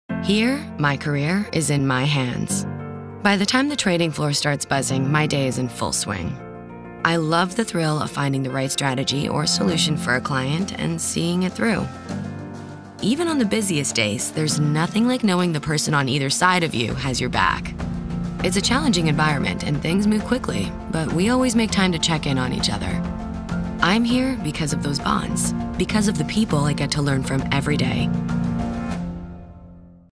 Day in the Life: Meet Julia
Meet Julia: Associate Trader and Goldman Sachs Fitness Center MVP. Watch our Day in the Life series for a look inside her experience at the firm and explore careers at Goldman Sachs.